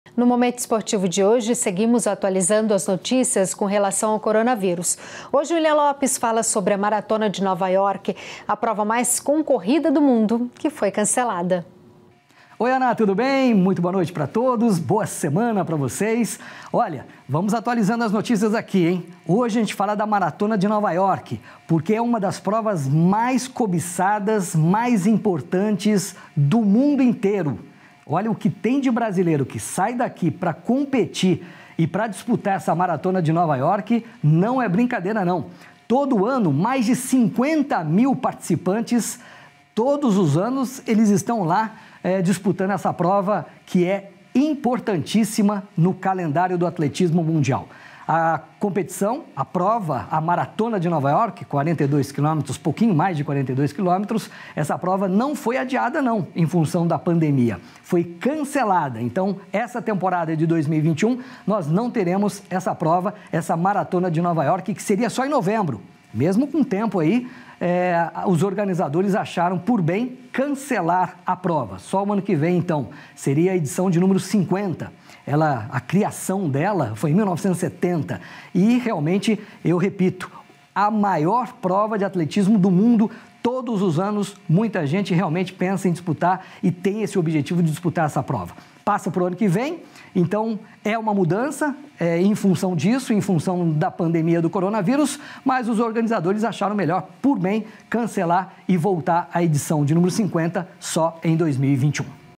Maratona de Nova York é cancelada - William - Jornal da Vida - 29/06/20
William Lopes fala sobre o cancelamento da Maratona de Nova York, a prova mais concorrida do mundo.